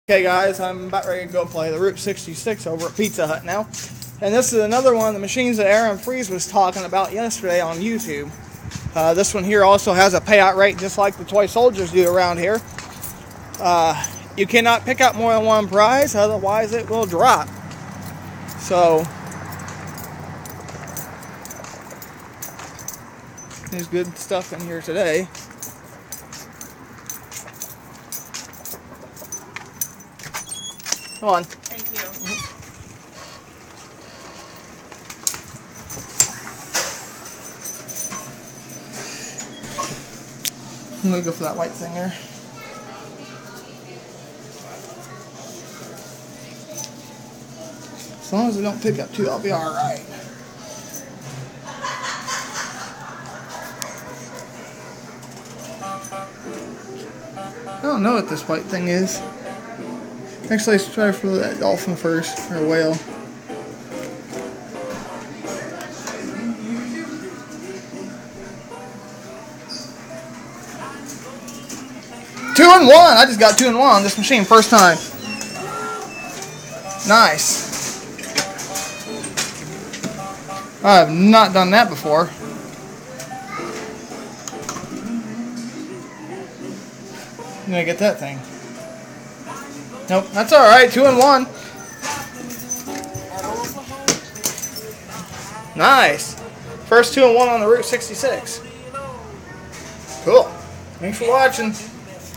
ROUTE 66 CLAW MACHINE @ PIZZA HUT MY FIRST 2 IN 1 WIN
TODAY I HEAD OVER TO MY LOCAL PIZZA HUT HERE AS U ARE ABOUT TO SEE A 2 IN 1 ON A ROUTE 66 MACHINE AND AS I POINT OUT BEFORE I ENTER PIZZA HUT THE CLAW ISNT STRONG ENOUGH TO WIN TWO AT ONCE THE CLAW WILL LET GO OF THEM BOTH SO AFTER MANY 2 IN NONE ATTEMPTS HERES THE FIRST